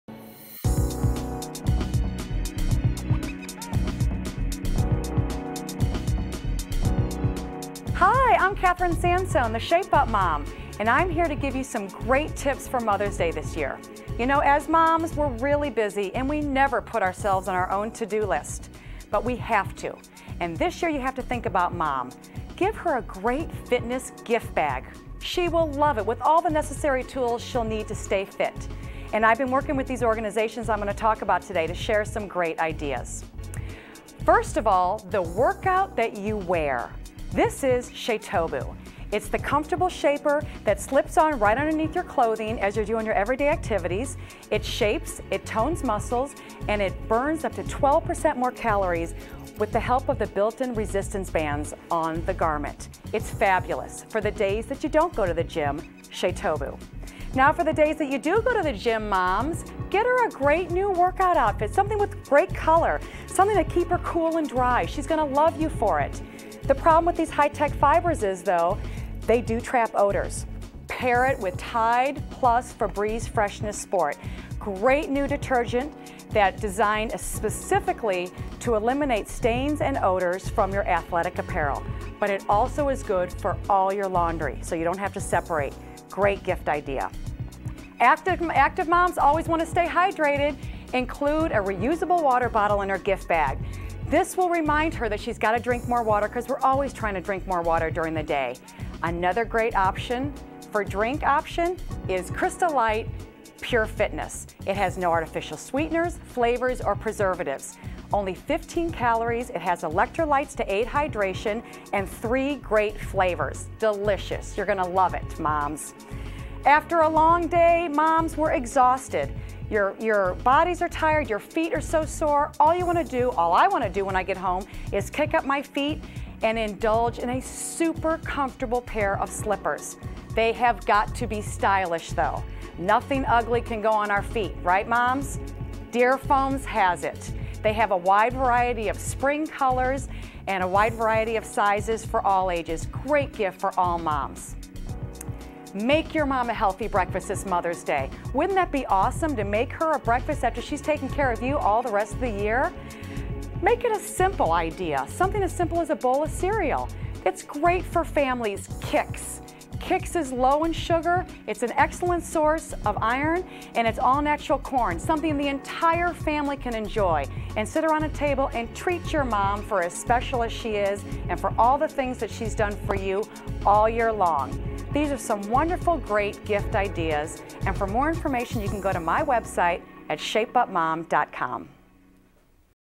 Mothers Day Gifts for the Active Mom
This Mothers Day, Kathryn Sansone, The Shape Up Mom shares some great gift ideas for the active mom in your life.  Video from ShāToBu, Tide, Crystal Light, Dearfoams, and Kix Cereal.